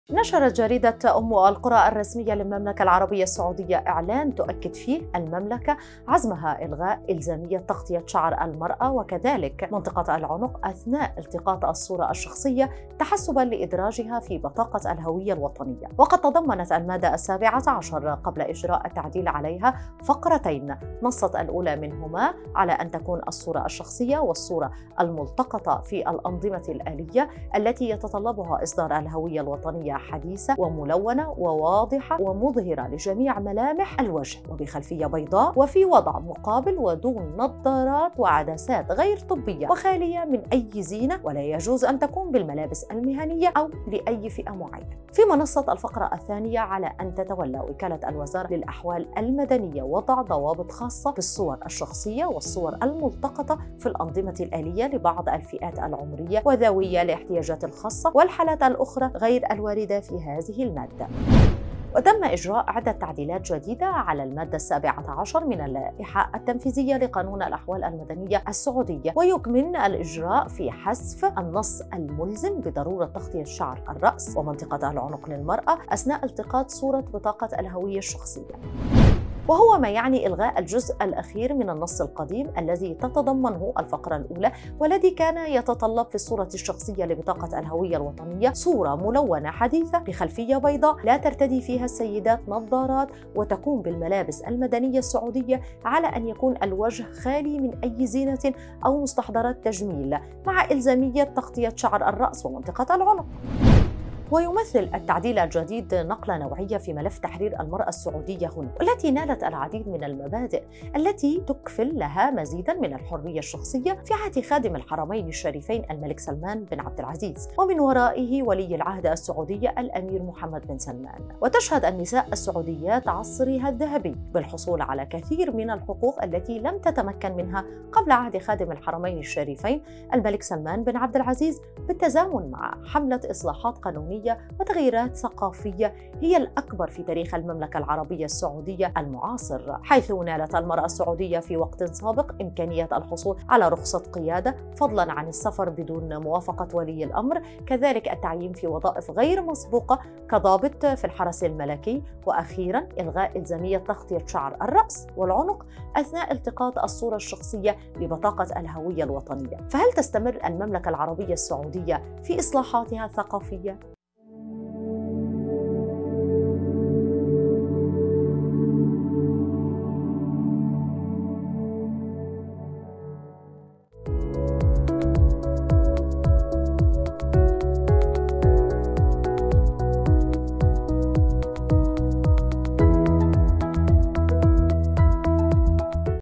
لماذا ألغت السعودية إلزامية تغطية شعر المرأة في البطاقة.. وما السبب الحقيقي لهذا القرار؟
خلال السنوات الماضية، شهدت المملكة العربية السعودية انفتاحًا كبيرًا، وتطورات كبيرة في المجال الحقوقي والحريات، والتي كان من بينها إلغاء إلزامية تغطية شعر المرأة في البطاقة.. فما القصة؟